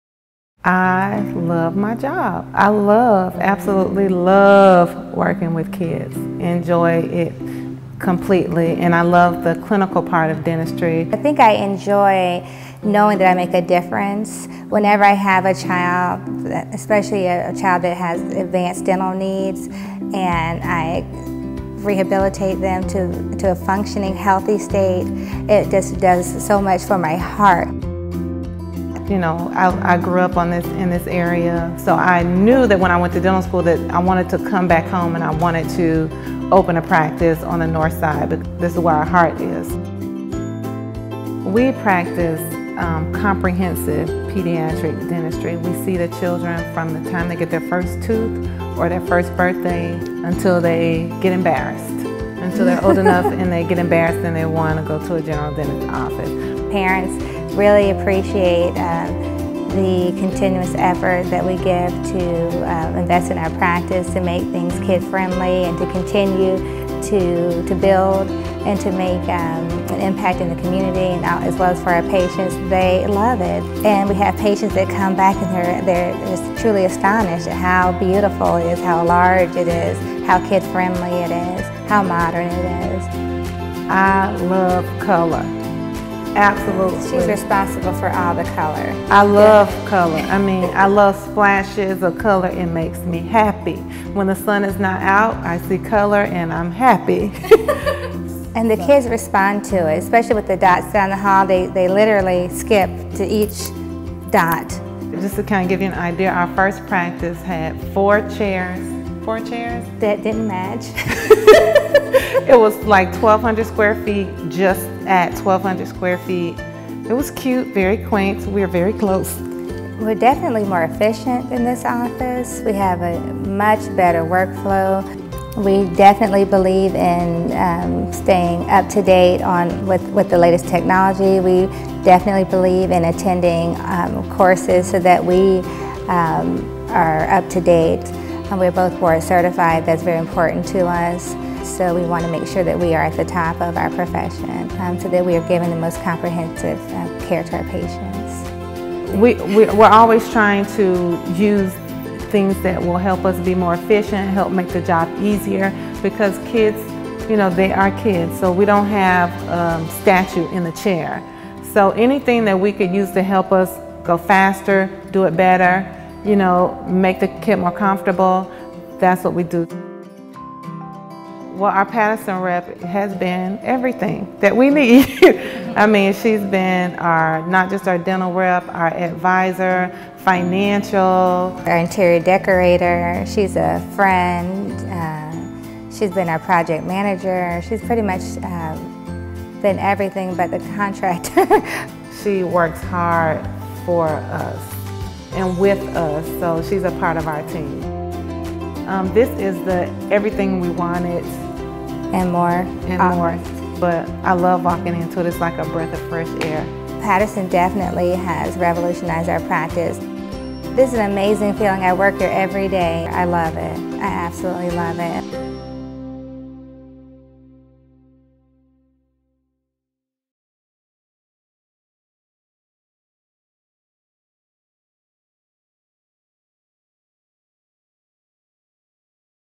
A Reason for Smiles
In Jacksonville, Florida, Patterson Dental helps Staci Suggs, DDS, and Tanya Wall, DDS, bring pediatric dental care to an area that didn't always have it. In the Jacksonville neighborhood Suggs grew up in, the new pediatric practice is close to the dentists' hearts.